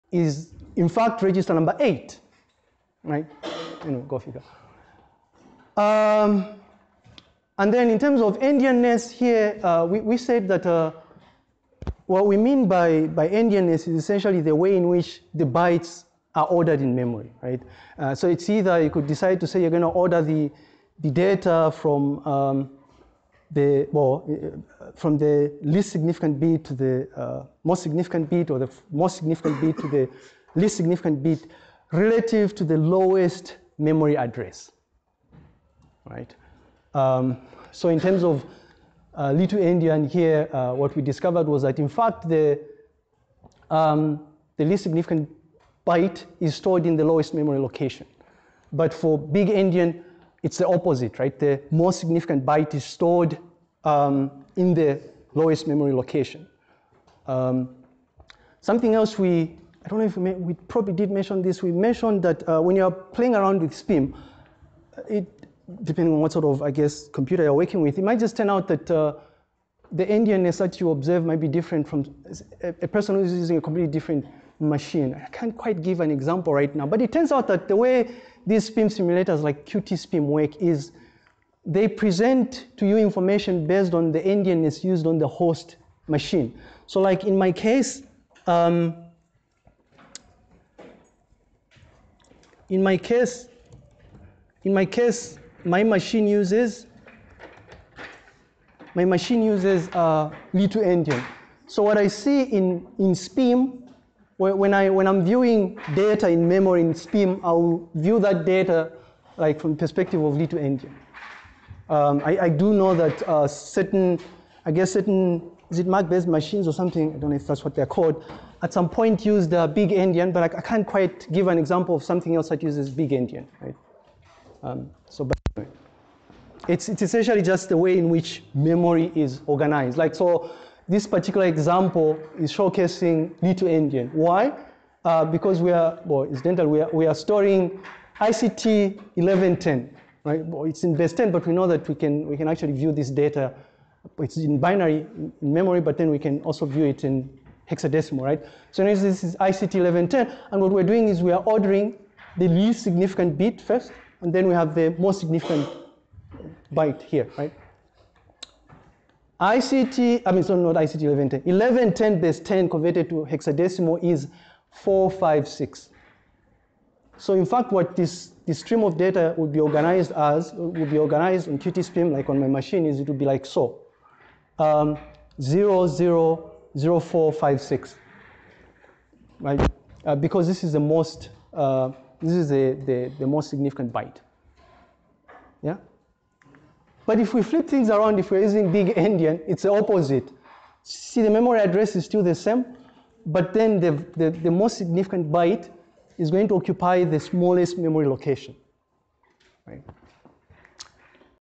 [Excerpts] MIPS Architecture: Little Endian vs Big Endian
Excerpt from Lecture Series 25 No. 1: Class Theory Test #04 Notes [1, 2, 3].

In class live outline of the differences between little endian and big endian